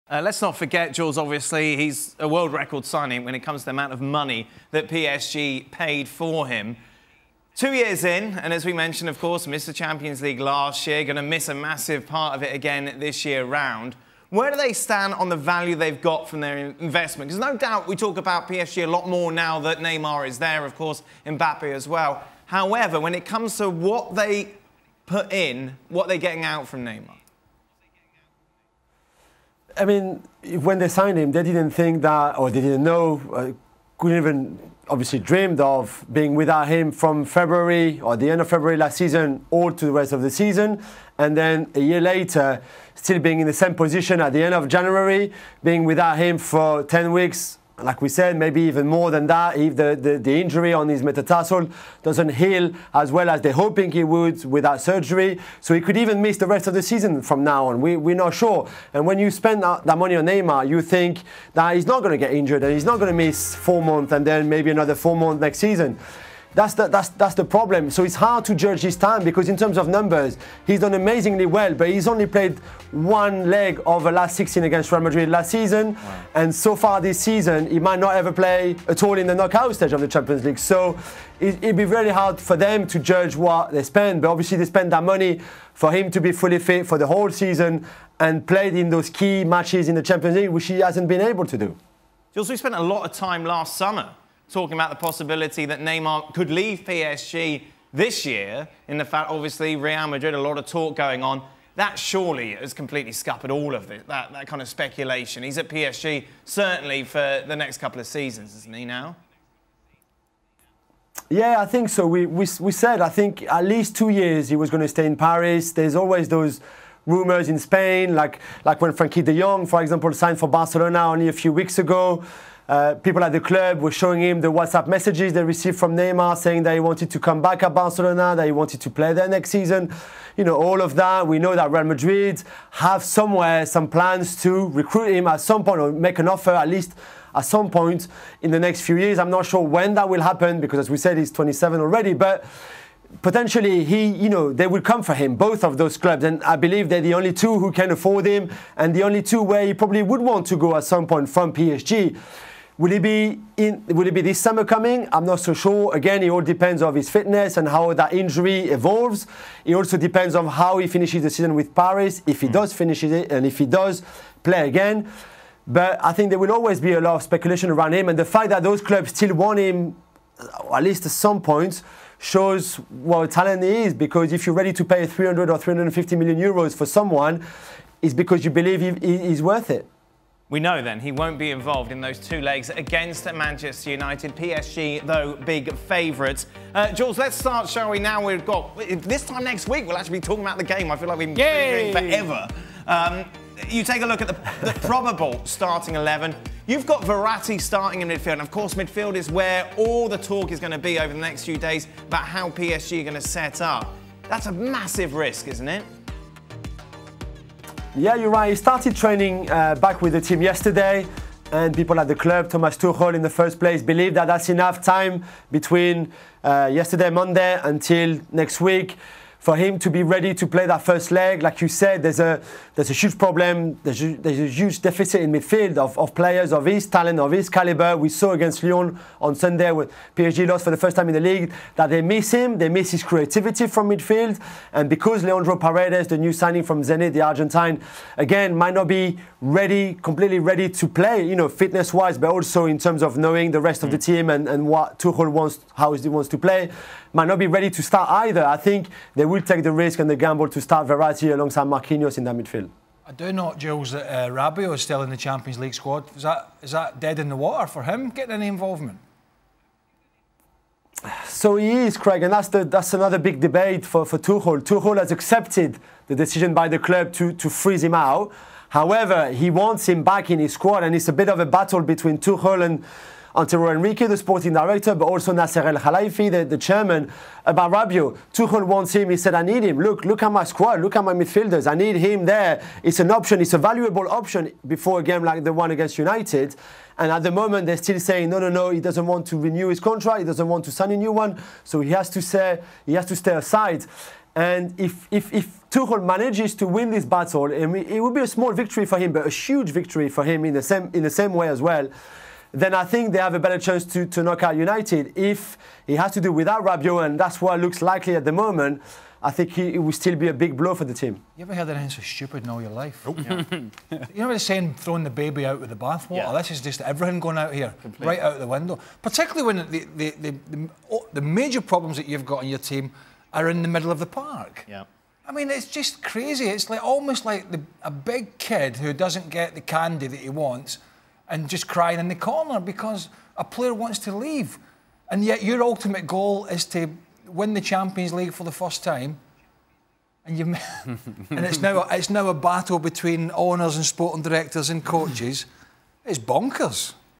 Has Neymar played his last game for PSG? | Champions League
Julien Laurens joins Dan Thomas, Shaka Hislop, Alejandro Moreno and Craig Burley of ESPN FC to discuss the future of Neymar, either at Paris Saint-Germain, Barcelona, Real Madrid or somewhere else. They also preview PSG vs. Manchester United in UEFA Champions League.